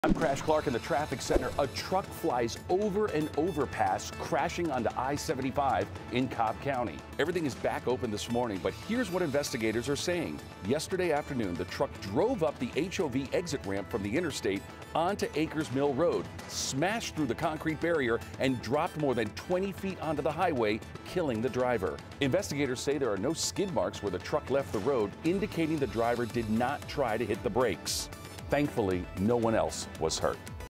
No skid marks where driver flew off overpass and crashed onto I-75
The driver died in the crash on Monday.